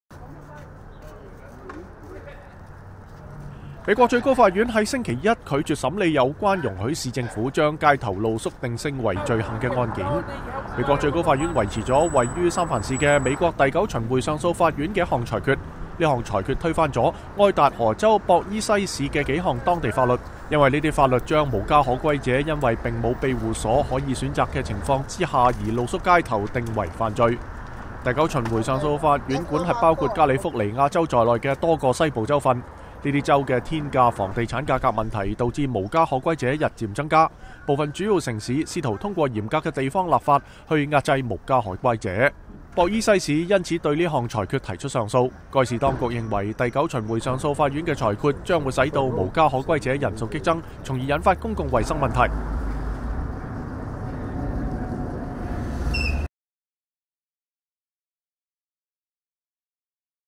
美國最高法院維持“街頭露宿”並非犯罪行為
美國最高法院在星期一拒絕審理有關容許市政府將街頭露宿定性為罪行的案件。美國最高法院維持了位於三藩市的美國第九巡迴上訴法院的一項裁決。該項裁決推翻了愛達荷州博伊西市(Boise)的幾項當地法律。這些法律將無家可歸者因為並無庇護所可以選擇的情況下露宿街頭而定為犯罪。